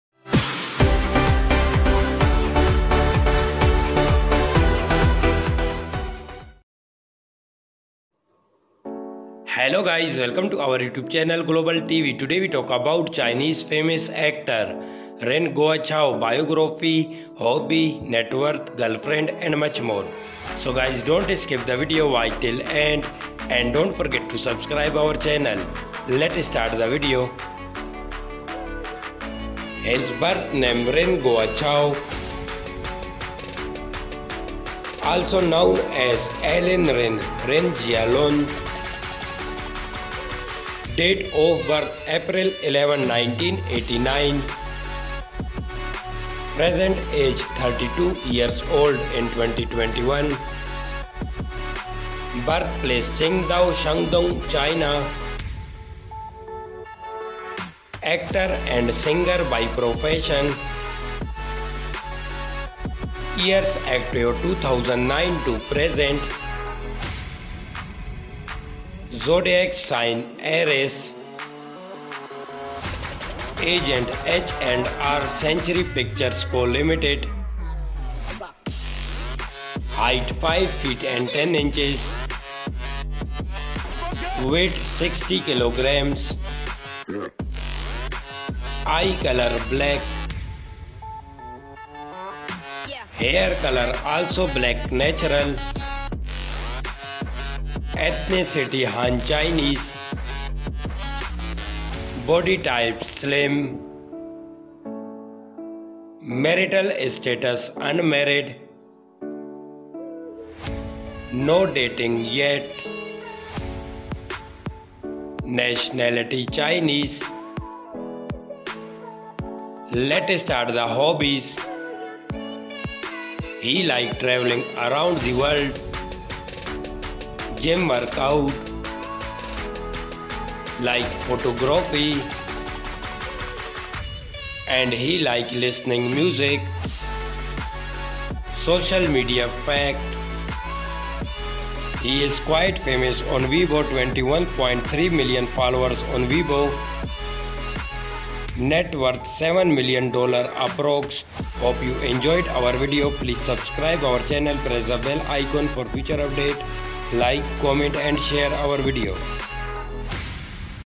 Ren Jia Lun Lifestyle (One and Only) Biography, Girlfriend, Income, Age, Height, Weight, Facts
Name: Allen Ren

Native name: 任国超

Given name: Ren Jia Lun

Also Known as: Prince;Leader;任嘉伦;Tony;Ren Jia Lun

Nationality: Chinese

Gender: Male

Born: April 11, 1989

Age: 32

Ren Jia Lun (Born: Ren Guo Chao, English name: Allen) is a Chinese actor born in Qingdao, Shandong, China. He was a professional table tennis player, who trained alongside Zhou Yu and Zhang Ke. However, he had to quit the sport due to injuries. Prior to beginning his acting career, Ren participated in Hunan TV's Super Boy and Korean Star. He was originally planned to debut in a Chinese-South Korean boy group but quit in 2014 due to unknown reasons.